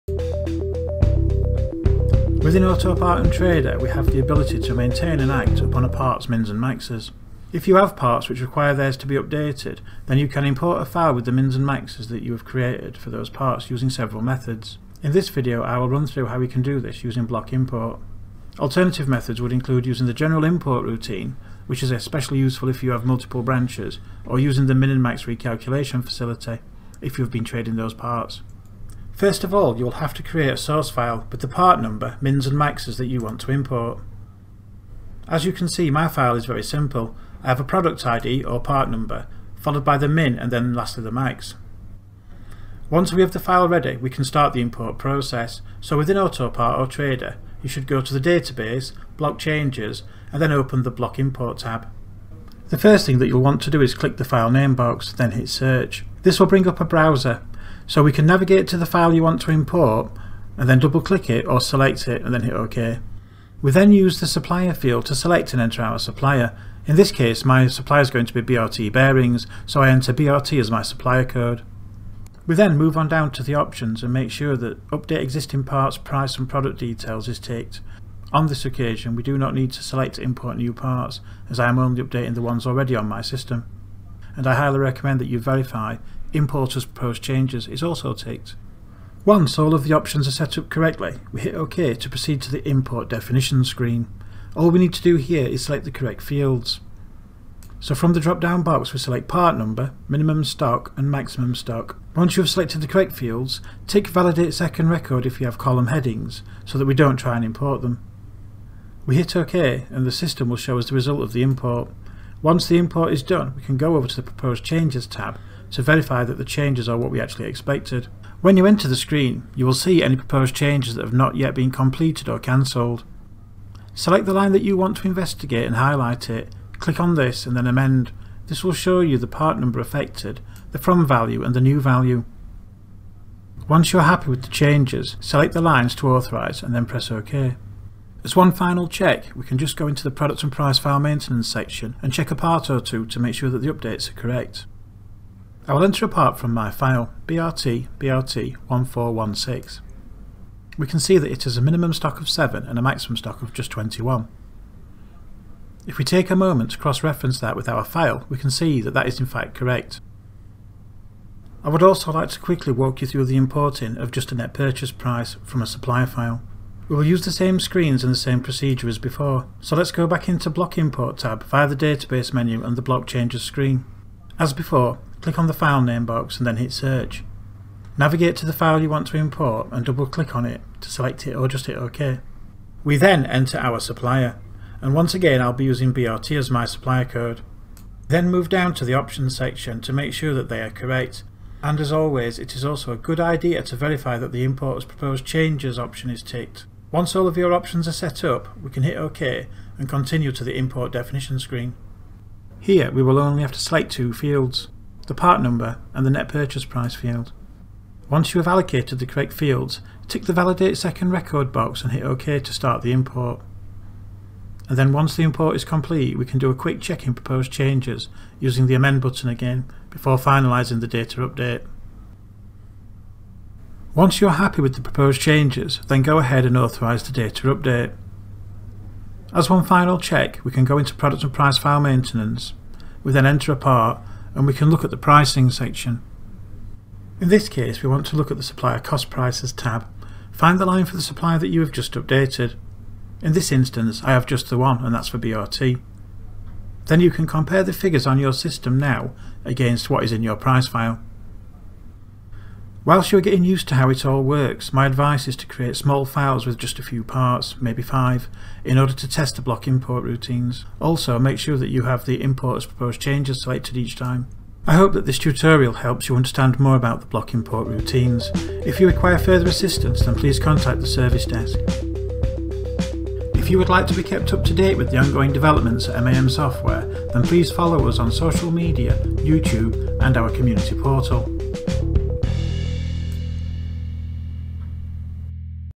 Autopart Price File Maintenance: Block Import Min, Max and NPP
In this tutorial, I will walk you through how to import or update your parts Net Purchase Price and the MINimum or MAXimum stock figures. If you keep these figures well maintained then stock ordering and pricing will much more accurate.

If you would like assistance with the block import then please contact our service desk. In upcoming videos, I will use other tools within Autopart which can help maintain your product and price file data.

If you would like more information about other MAM products please

Music